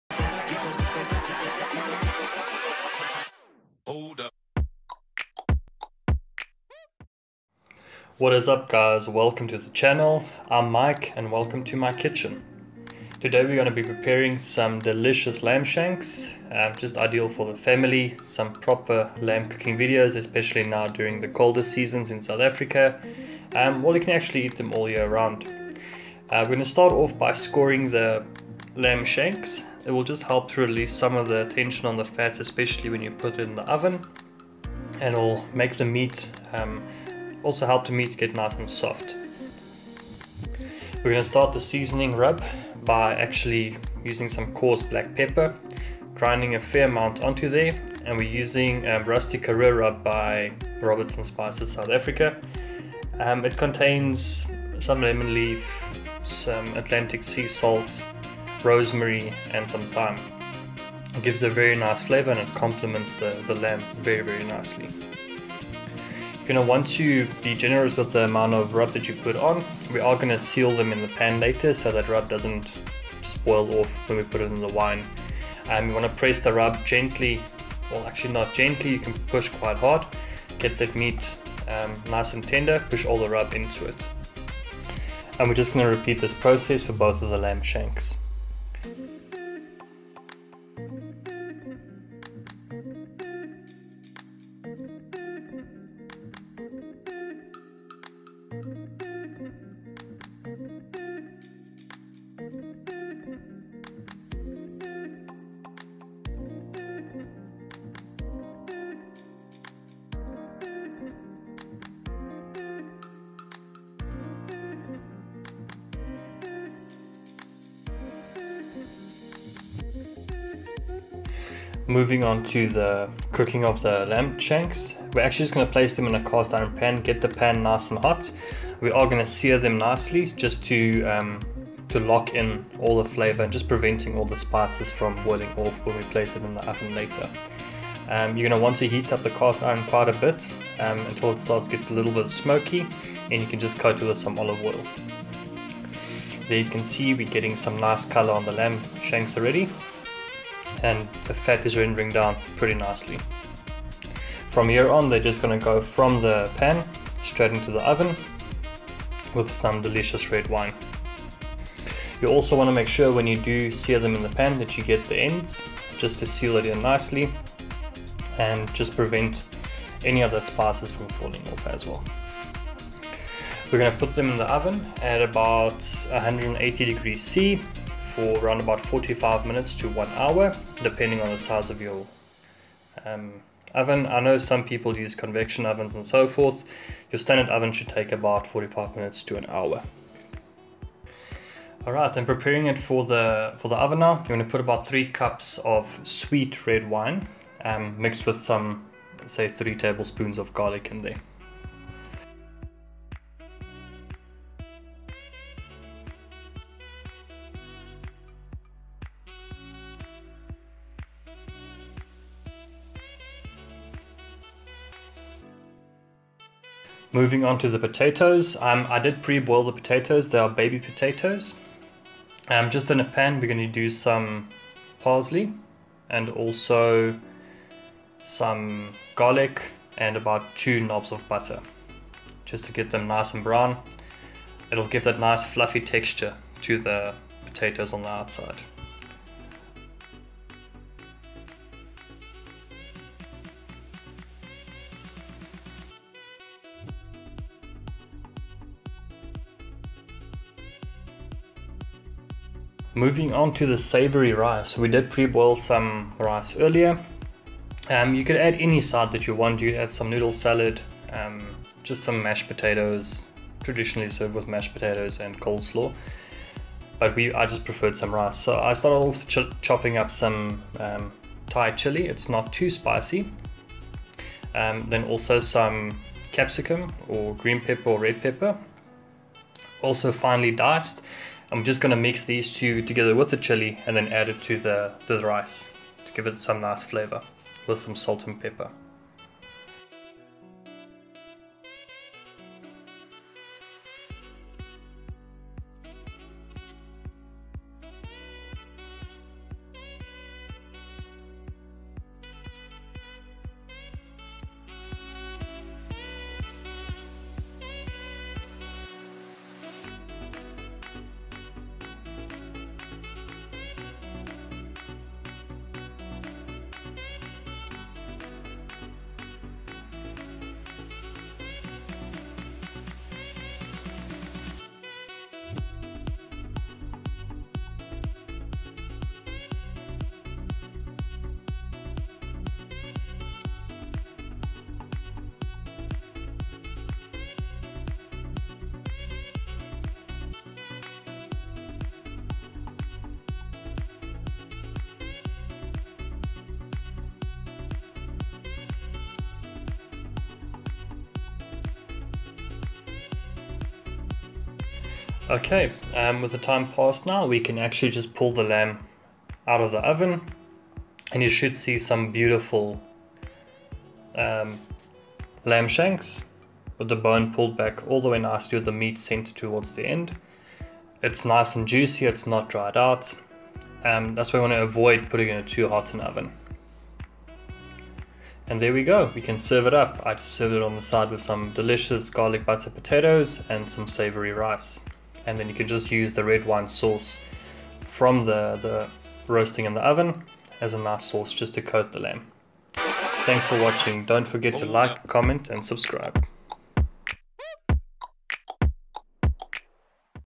Lamb Shank | Red Wine Sauce | Lockdown Roasting
Boiled lamb ...For Who ? Kick it up a notch with some red wine , garlic and you just might make it through the lockdown.

In Todays Video were preparing delicious Lamb Shanks, seared in the pan and braized in some sweet red wine sauce. Served alongside golden , buttery baby potatoes and savoury rice.

Ingredients:

lamb Shanks
Coarse Sea salt
Coarse Black pepper
Rosemary
Lemon pepper
Garlic
Thyme
Olive Oil